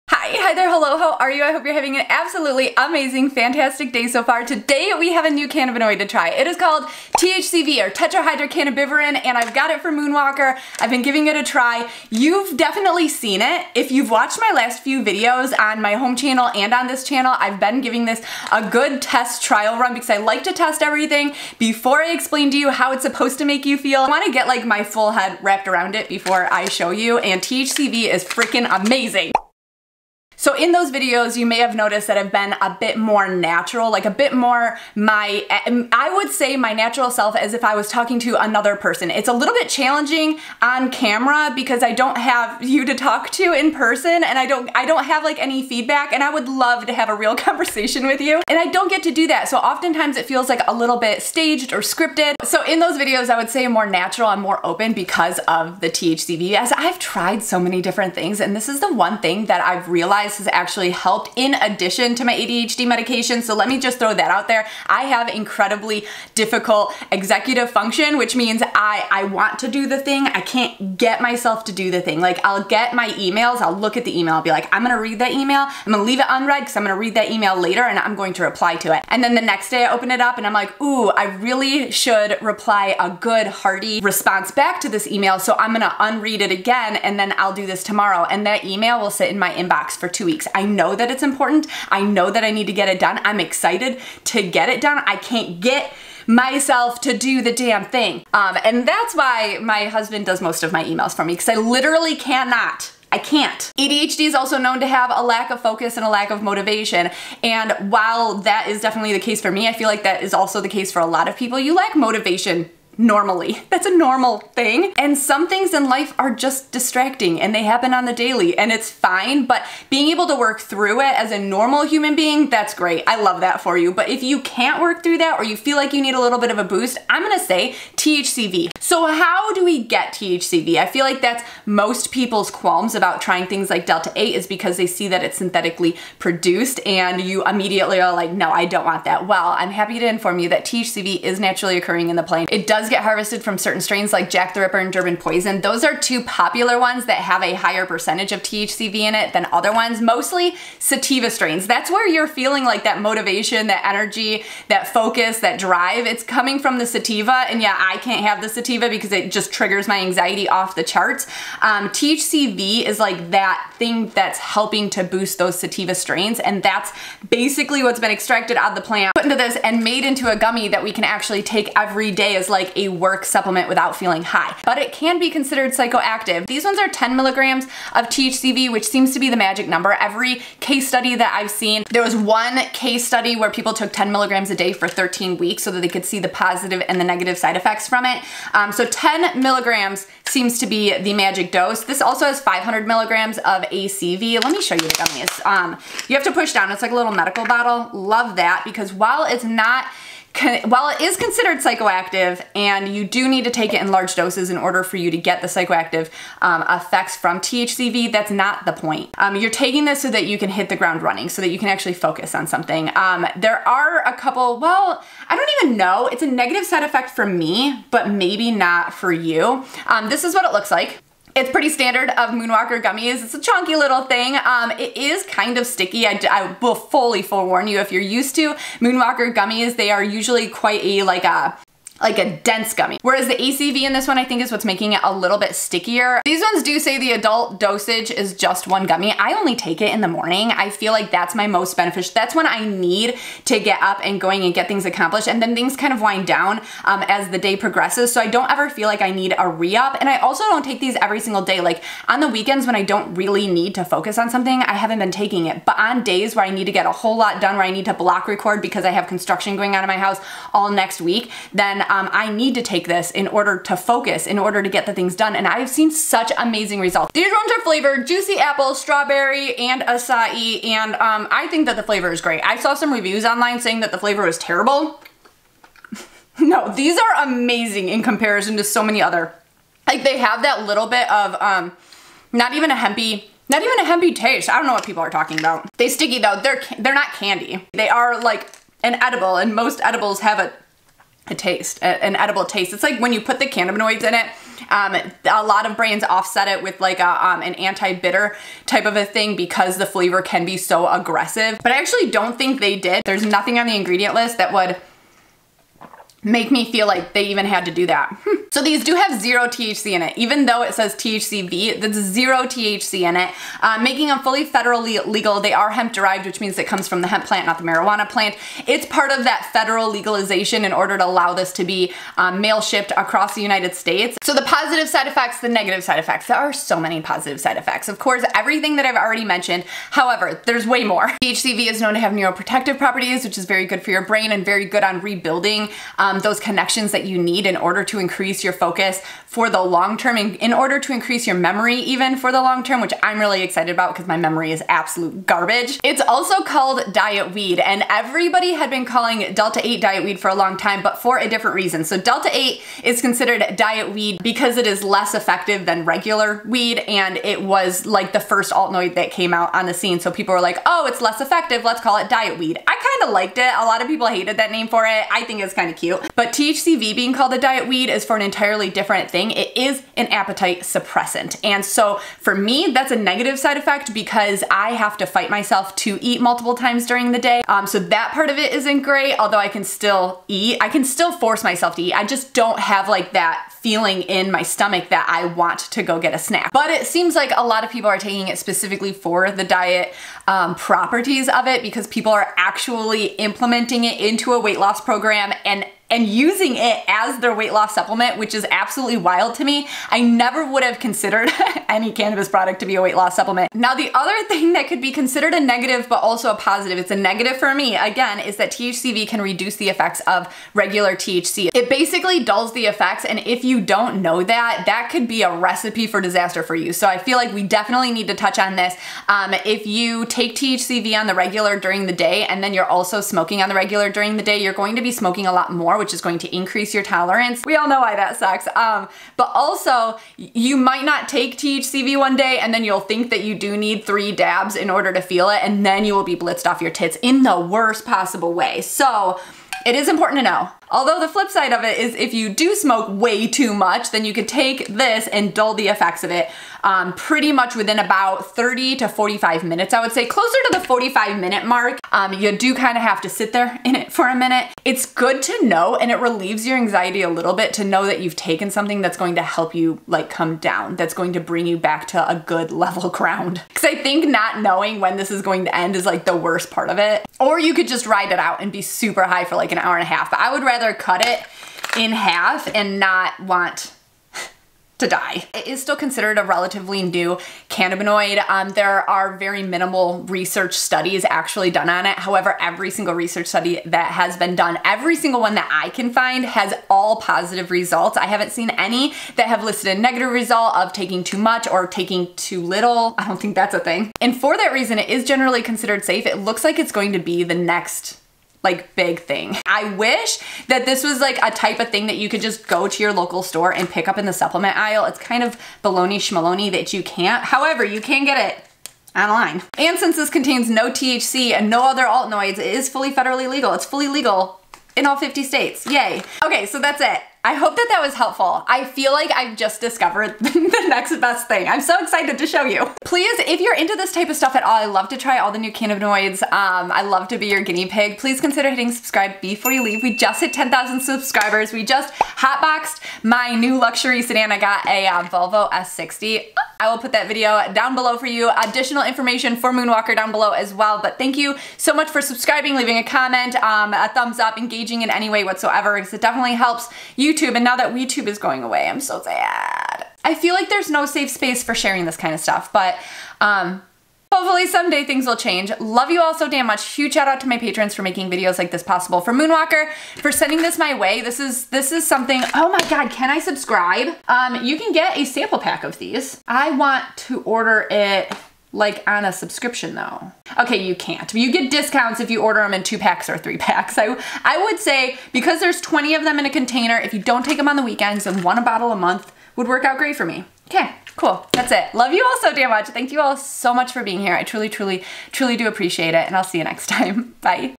THCV? WEVE GOT A NEW CANNABINOID FROM MOONWLKR! | everything we need to know
What have you heard about THCV? Have you tried it yet?  Moonwlkr sent me over a bottle of their THCV+ACV gummies and Ive been testing them out for you :)  Thank you for letting me be your guinea pig!

Moose Labs MouthPeace Silicone Filter ~CHRISSY15
Amazon Faves Storefront

*TIMESTAMPS*
Hi friends! 0:00
Where does it come from? 2:08
How does it feel? 2:54
Is it legal though? 5:08
The side effects 5:24
Bye friends 8:36